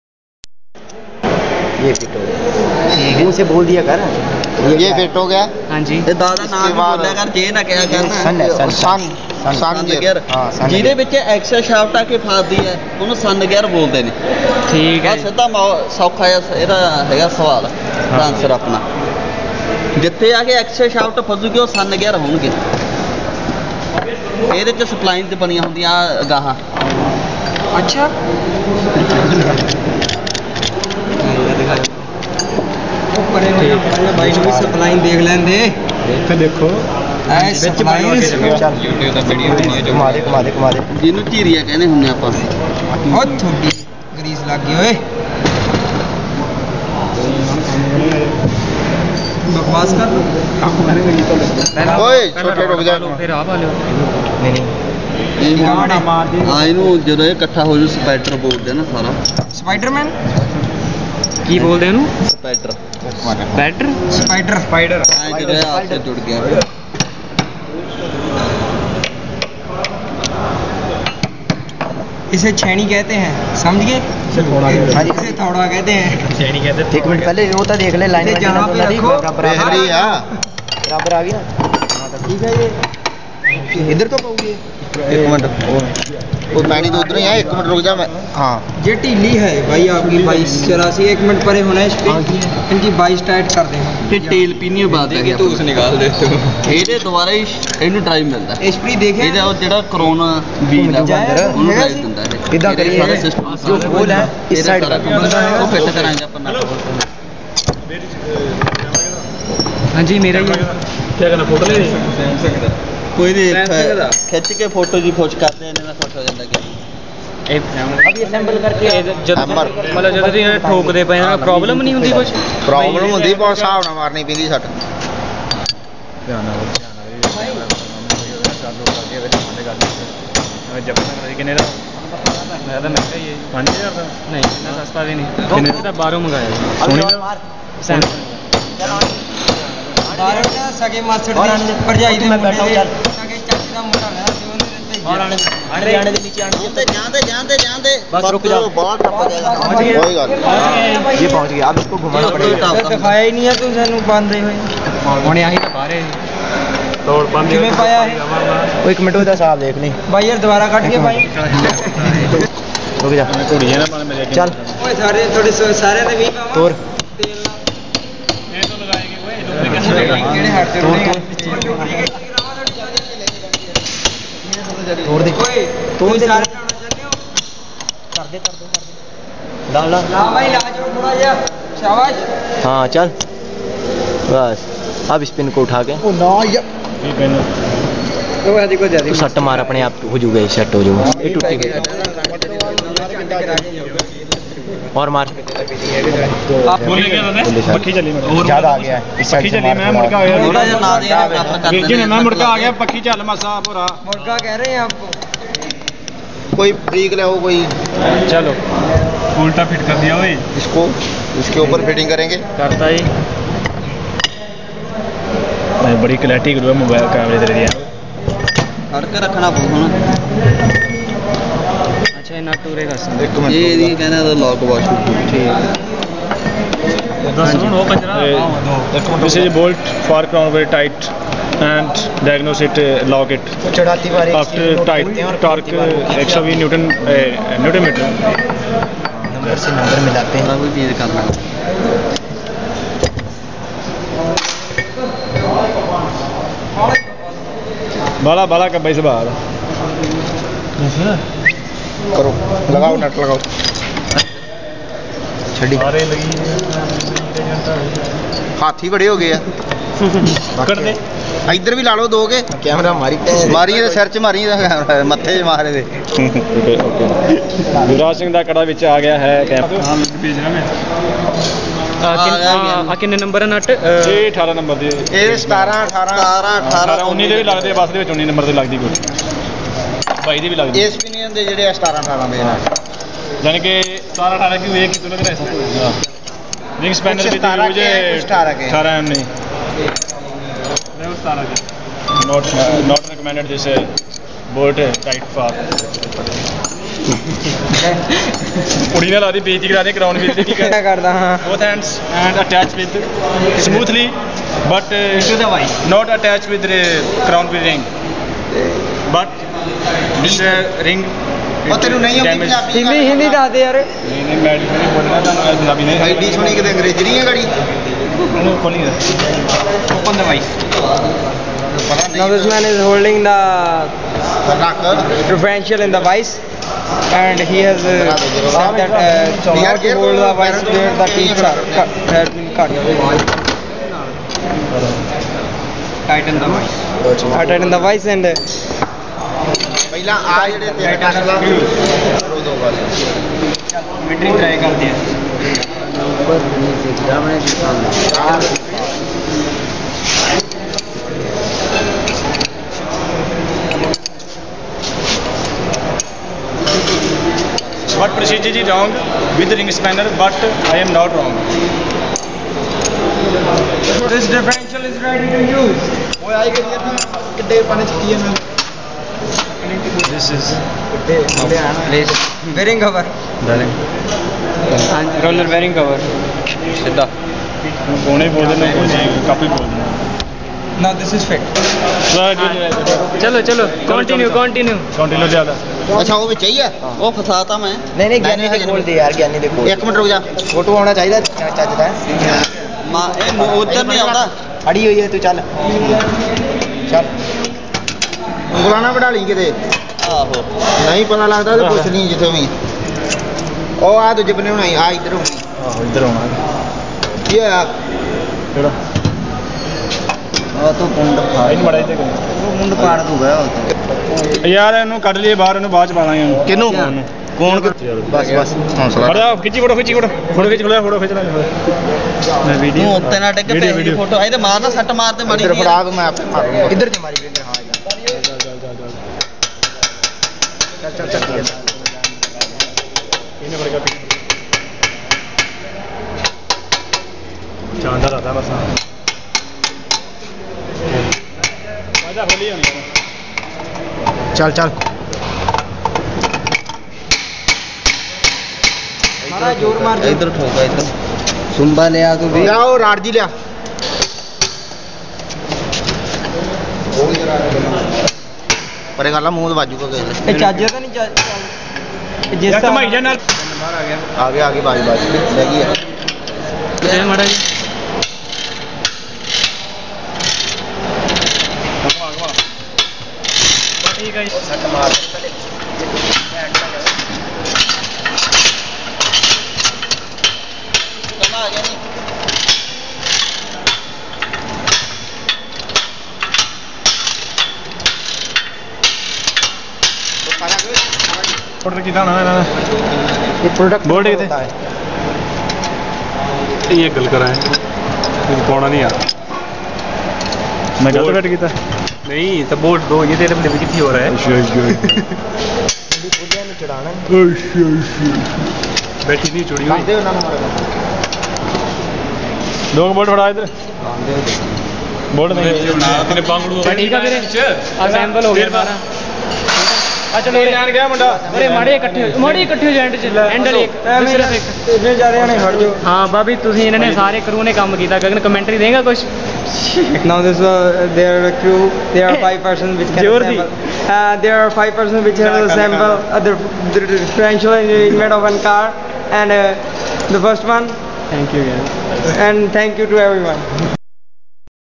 ASSEMBLING A DIFFERENTIAL of a car - at advance training institute , Ludhiana
Assembling diffrential  at A.T.I Ludhiana in  with complete description of each part of a simple differential. Function of each component being thoroughly explained . working of differential and its use in automobiles in cleared in a simple manner.